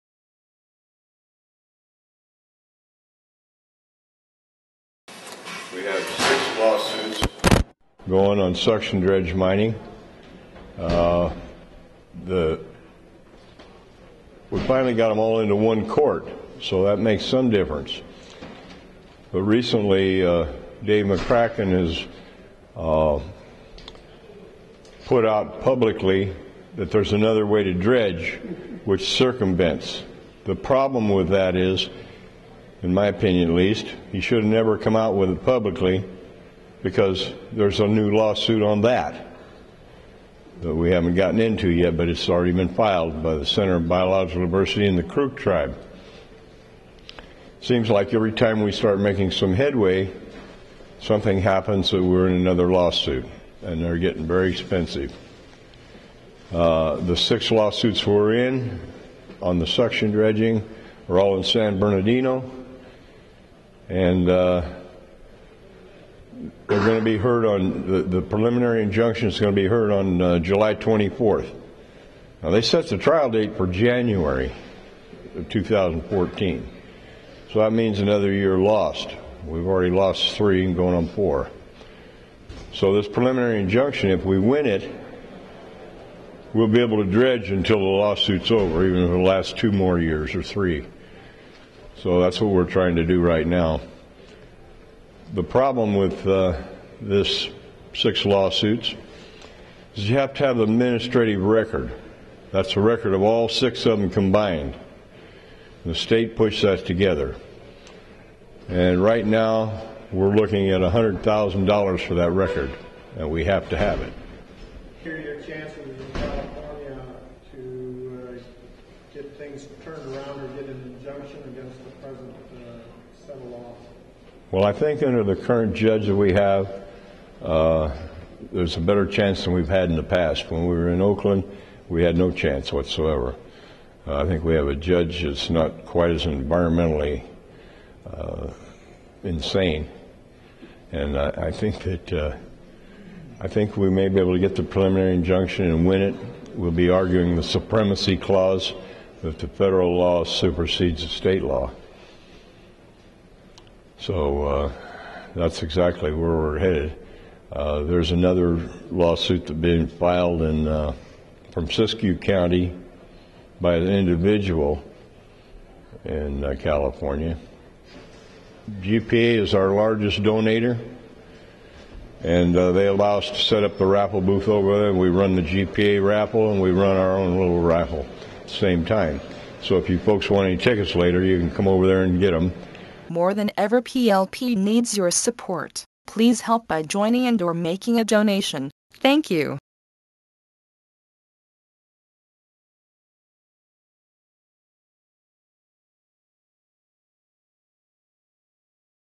Jerry Hobbs talks on California Mining Issues
Small-Scale mining is under assault and PLP (Public Lands For The People) is leading the fight in court.  Jerry talks on what PLP is doing and the questions involved in the presnt court case.  From GPAA Gold Show Puyallup, Washington